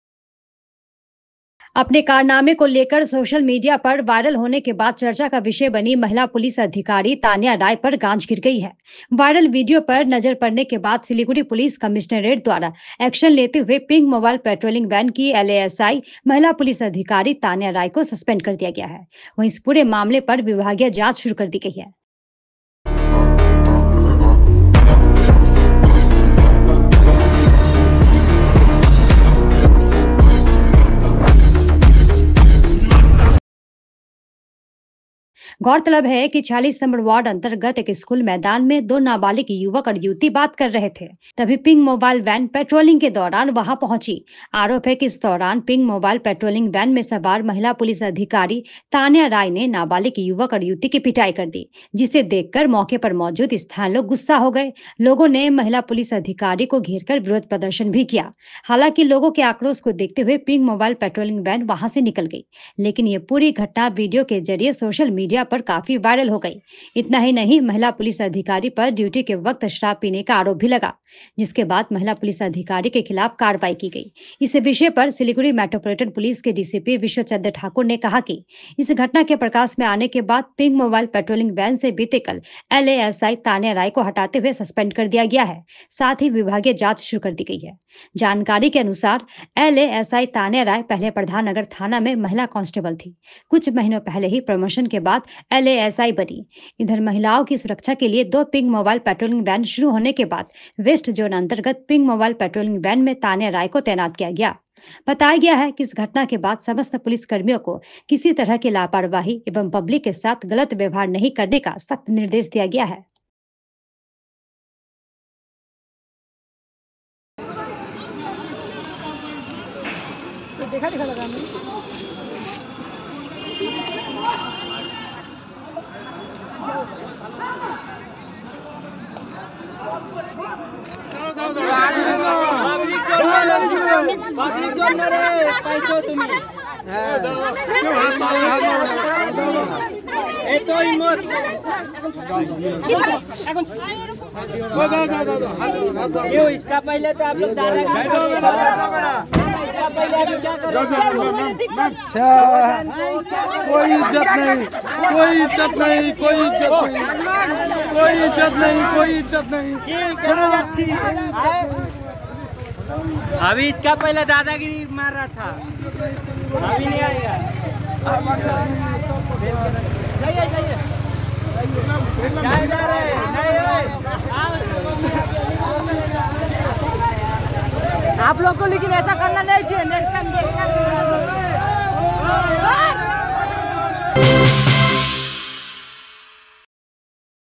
Woman police officer suspended for alleged assault of minors and being intoxicated on duty (Hindi)
Woman police officer suspended for alleged assault of minors and being intoxicated on duty in Siliguri (Hindi)